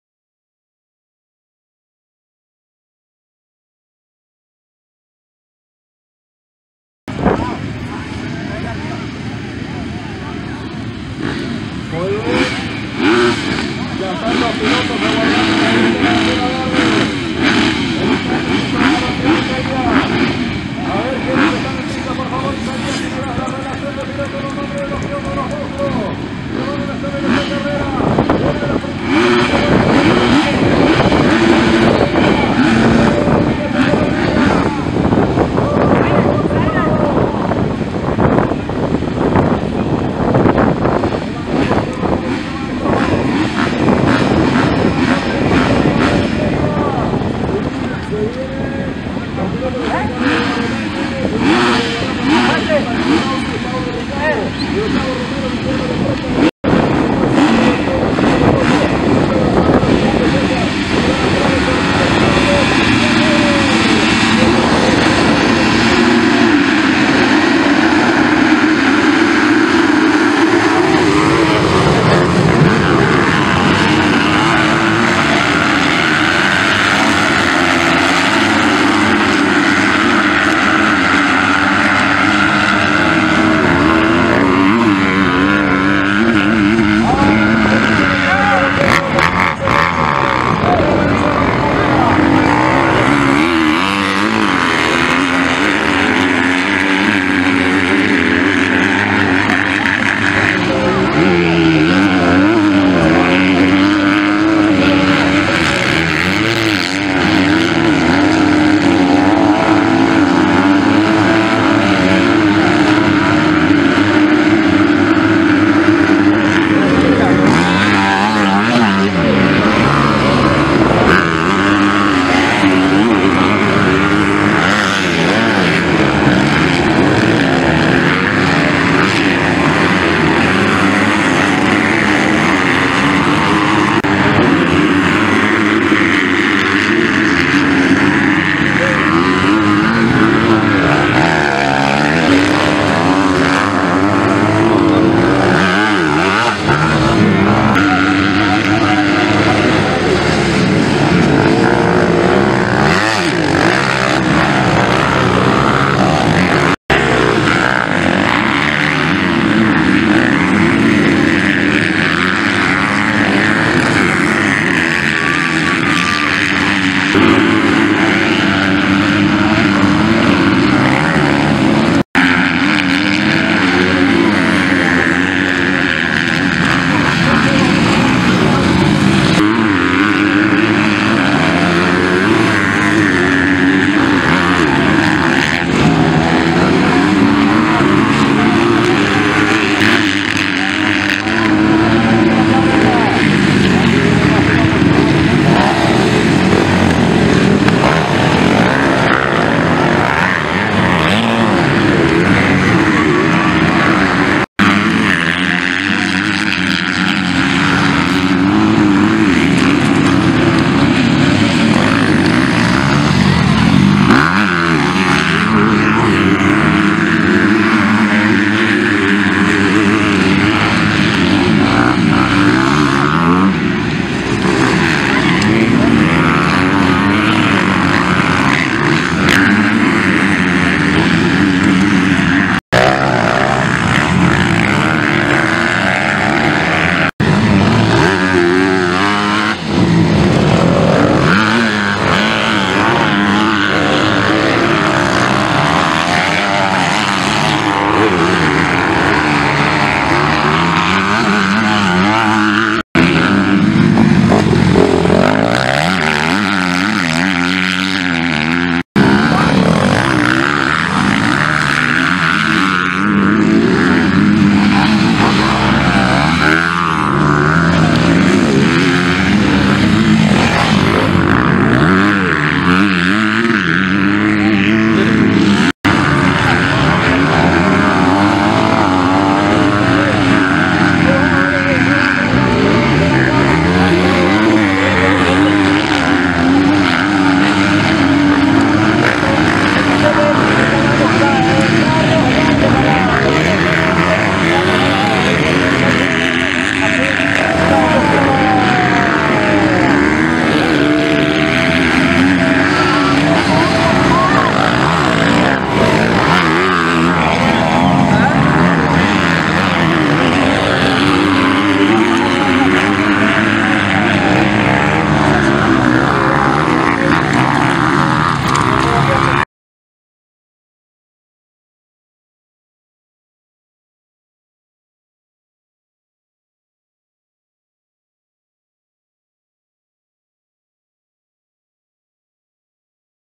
Vitória de Carlos Eduardo na VX2 em La Paloma
2º Etapa do Paraguaio de Velocross

Gate
#05 Gabriel Mariani
#12 Adenilson Vilanova
#14 Pedro Henrique
#20 Gustavo Romero
#25 Lucas Gasparello
#58 Diego Yanahiara
#87 Alvaro Ortiz
#153 Douglas Schweig
#206 Charlos Luft
#221 Yony Vilanova
#275 Diomar Júnior
#277 David Rojas
#313 Jean Meurer
#317 Carlos Eduardo
#777 Murilo Sedran
#976 Alejandro Blanco